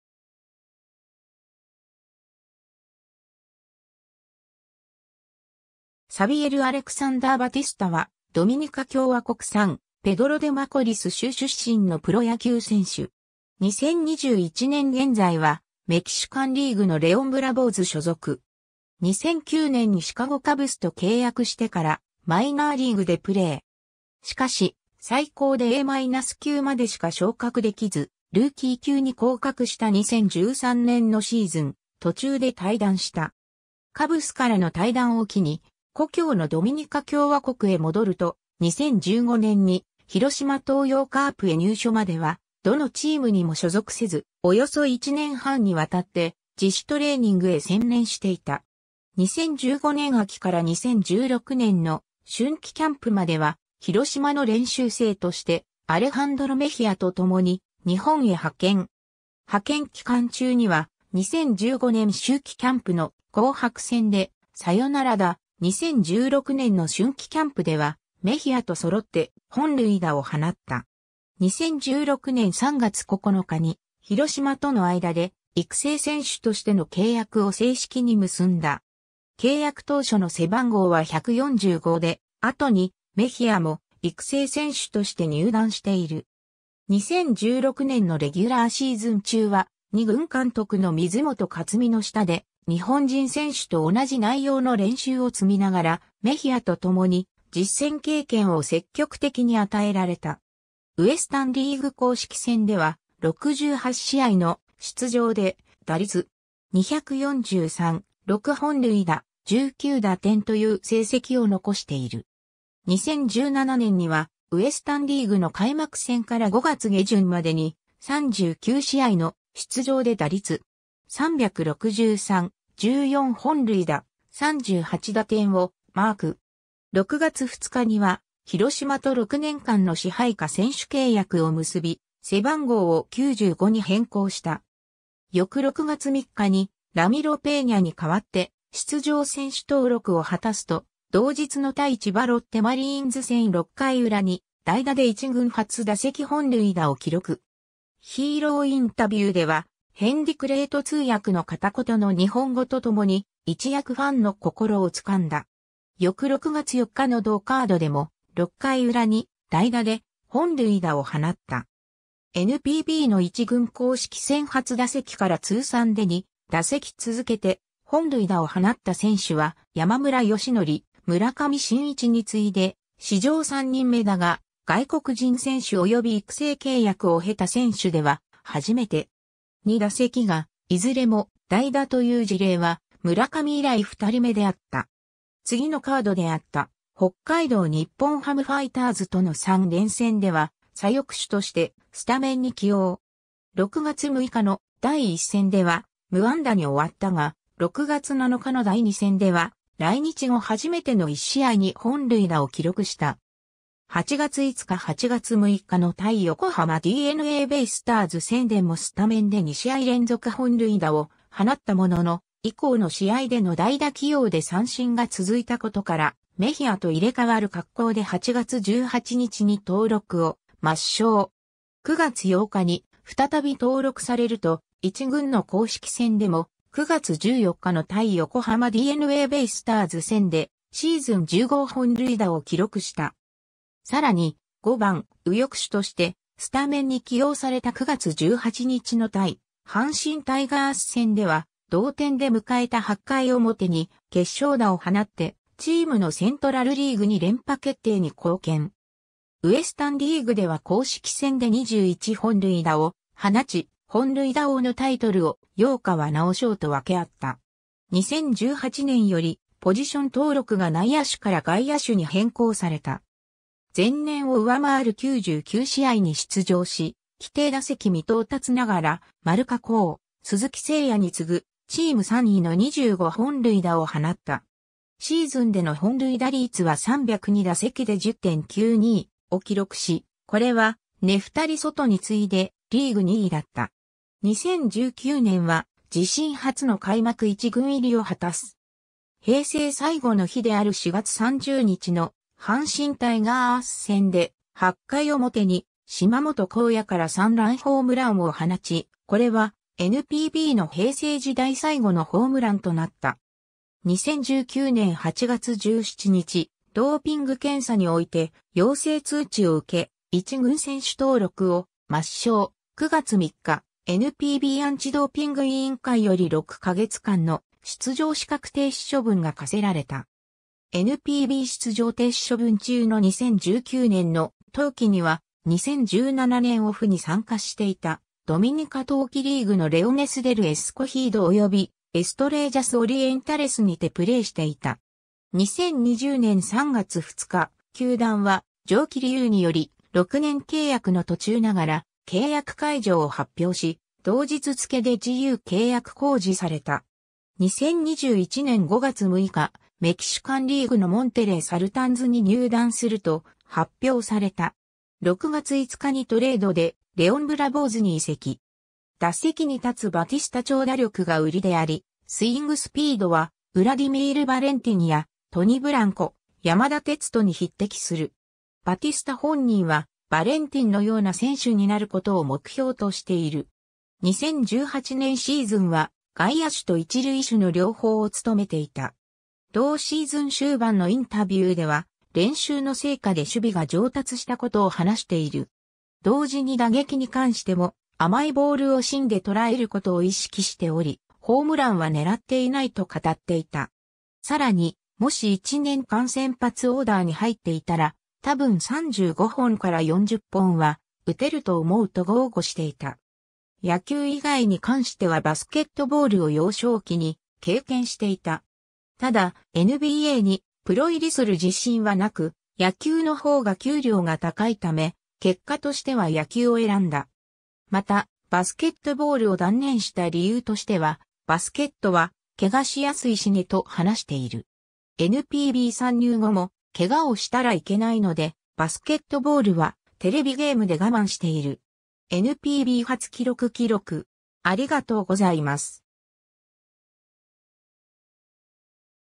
サビエル・バティスタ
サン・ペドロ・デ・マコリス州出身の人物
サビエル・アレクサンダー・バティスタ（Xavier Alexander Batista、1992年1月18日 - ）は、ドミニカ共和国サン・ペドロ・デ・マコリス州出身のプロ野球選手（一塁手、左翼手）。
2021年現在は、メキシカンリーグのレオン・ブラボーズ所属。
 2009年にシカゴ・カブスと契約してからマイナーリーグでプレー。
しかし、最高でA-級までしか昇格できず、ルーキー級に降格した2013年のシーズン途中で退団した。
 カブスからの退団を機に故郷のドミニカ共和国へ戻ると、2015年に広島東洋カープへ入所まではどのチームにも所属せず、およそ1年半にわたって、自主トレーニングへ専念していた。
 2015年秋から2016年の春季キャンプまでは、広島の練習生として、アレハンドロ・メヒアとともに日本へ派遣。
派遣期間中には、2015年秋季キャンプの紅白戦でサヨナラ打、2016年の春季キャンプではメヒアと揃って本塁打を放った。
 2016年3月9日に、広島との間で、育成選手としての契約を正式に結んだ。
契約当初の背番号は145で、後にメヒアも育成選手として入団している。
 2016年のレギュラーシーズン中は、二軍監督の水本勝己の下で日本人選手と同じ内容の練習を積みながら、メヒアと共に実戦経験を積極的に与えられた。
ウエスタン・リーグ公式戦では、68試合の出場で、打率.243、6本塁打、19打点という成績を残している。
 2017年には、ウエスタン・リーグの開幕戦から5月下旬までに、39試合の出場で打率.363、14本塁打、38打点（いずれもリーグ1位）をマーク。
6月2日には、広島と6年間の支配下選手契約を結び、背番号を95に変更した。
翌6月3日にラミロ・ペーニャに代わって出場選手登録を果たすと、同日の対千葉ロッテマリーンズ戦（MAZDA Zoom-Zoom スタジアム広島）6回裏に代打で一軍初打席本塁打を記録。
ヒーローインタビューではヘンディ・クレート通訳の片言の日本語とともに一躍ファンの心を掴んだ。
翌6月4日の同カードでも、6回裏に代打で本塁打を放った。
NPBの一軍公式戦初打席から通算で2打席続けて本塁打を放った選手は、山村善則・村上信一に次いで史上3人目だが、外国人選手および育成契約を経た選手では初めて。
2打席がいずれも代打という事例は、村上以来2人目であった。
次のカードであった北海道日本ハムファイターズとの3連戦（札幌ドーム）では、左翼手としてスタメンに起用。
6月6日の第1戦では無安打に終わったが、6月7日の第2戦では、来日後初めての1試合2本塁打を記録した。
8月5日・8月6日の対横浜DeNAベイスターズ戦（横浜スタジアム）でもスタメンで2試合連続本塁打を放ったものの、以降の試合での代打起用で三振が続いたことから、メヒアと入れ替わる格好で8月18日に登録を抹消。
9月8日に再び登録されると、一軍の公式戦でも、9月14日の対横浜DeNAベイスターズ戦（マツダスタジアム）でシーズン10号本塁打を記録した。
さらに、「5番・右翼手」としてスタメンに起用された9月18日の対阪神タイガース戦（阪神甲子園球場）では、同点で迎えた8回表に決勝打を放ってチームのセントラル・リーグ2連覇決定に貢献。
ウエスタン・リーグでは公式戦で21本塁打を放ち、本塁打王のタイトルを陽川尚将と分け合った。
 2018年よりポジション登録が内野手から外野手に変更された。
前年を上回る99試合に出場し、規定打席未到達ながら丸佳浩、鈴木誠也に次ぐチーム3位の25本塁打を放った。
シーズンでの本塁打率は302打席で10.92を記録し、これはネフタリ・ソトに次いでリーグ2位だった。
 2019年は自身初の開幕一軍入りを果たす。
平成最後の日である4月30日の阪神タイガース戦で、8回表に島本浩也から3ランホームランを放ち、これはNPBの平成時代最後のホームランとなった。
 2019年8月17日、ドーピング検査において陽性通知を受け、一軍選手登録を抹消、9月3日、NPBアンチ・ドーピング委員会より6か月間の出場資格停止処分が科せられた。
NPB出場停止処分中の2019年の冬季には、...